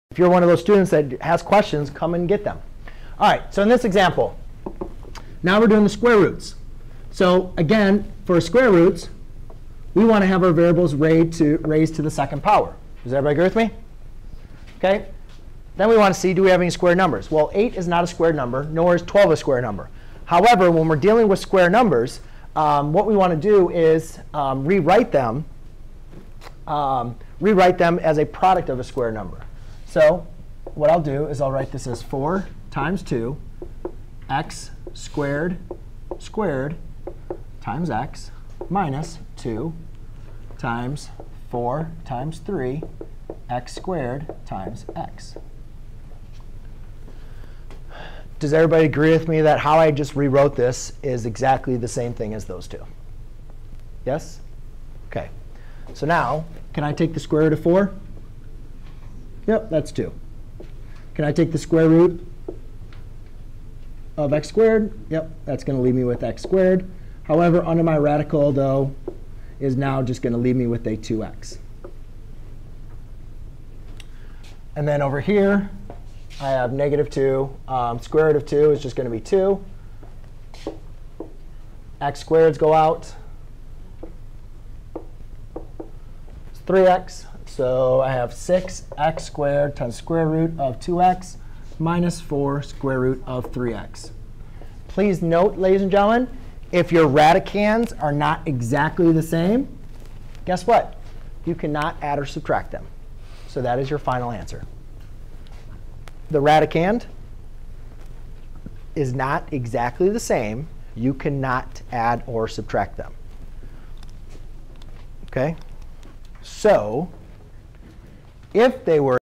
Simplify and add two radical expressions
👉 Learn how to add or subtract radicals. A radical is a number or an expression under the root symbol. Radicals can only be added or subtracted if the numbers or expressions under the roots are the same for all terms. To add or subtract radicals, we reduce/simplify the radicals and then add/subtract the coefficients and retain the root part.